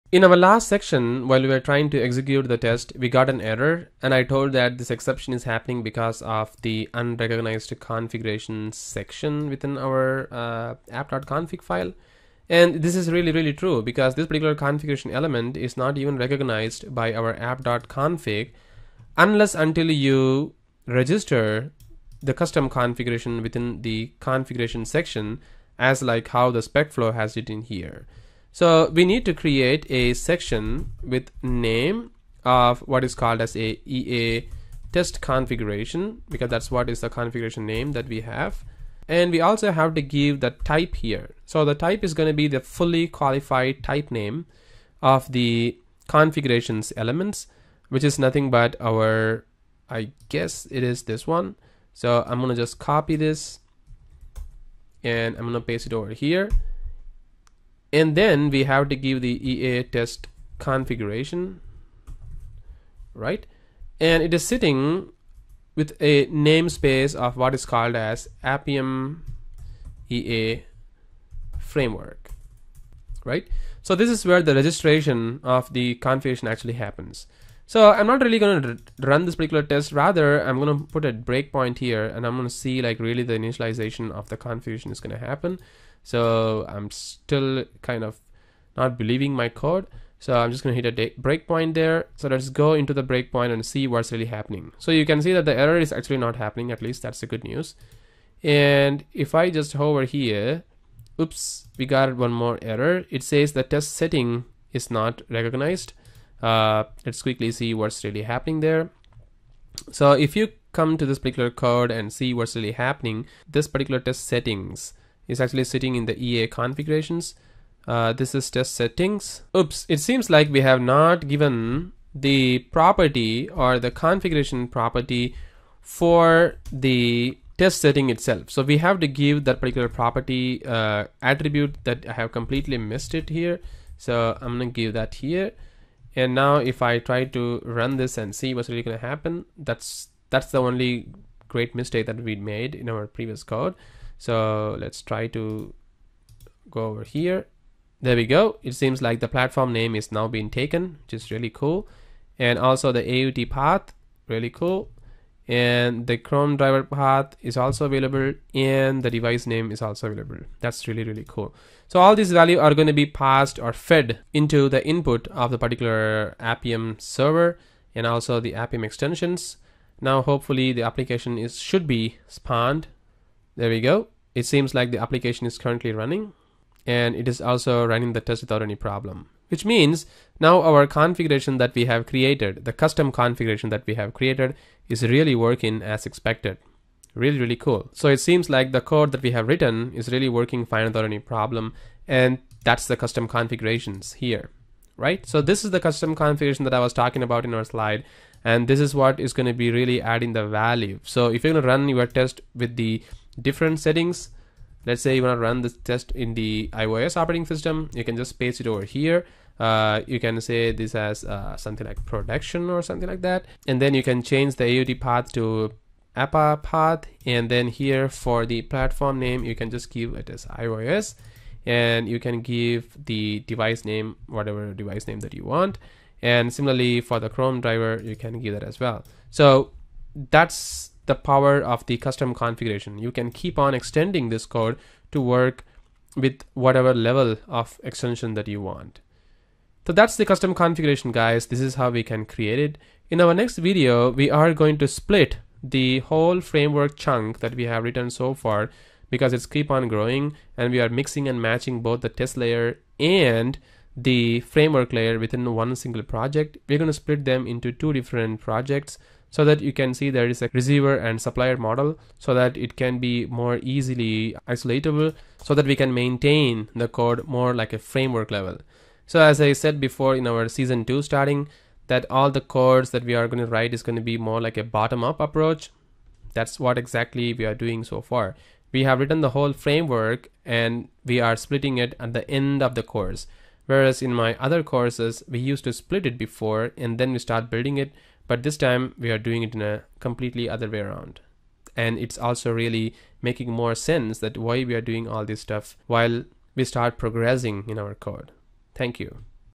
Custom configuration Implementation for Appium automation with iOS and Android
In this video, we will discuss about custom configuration with C# which can be used within our Appium framework development

This is the first ever course of its type which covers automation of Ionic and Xamarin application automation with C# language binding.

This video tells the final implementation of two Seasons in the course!

This video is part of the complete course available in Udemy